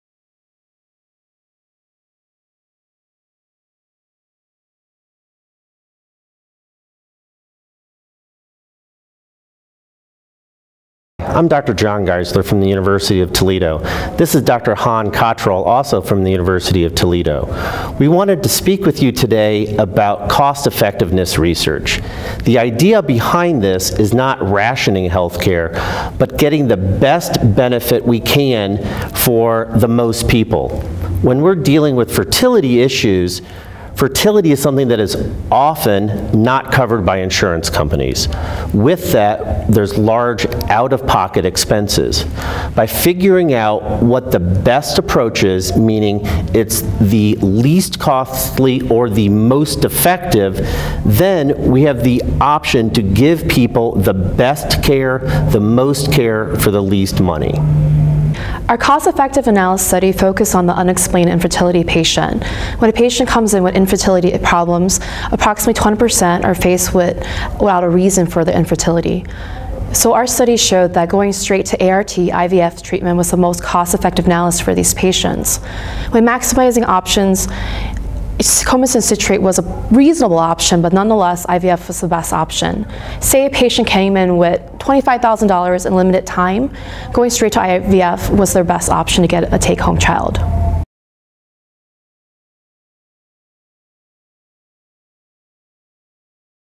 IVF Most Cost-Effective Treatment for Unexplained Infertility
John P. Geisler, MD, and Hahn Cottrell, MD, discuss their research that found skipping straight to in vitro fertilization was the most cost-effective route to having a baby in patients with unexplained infertility.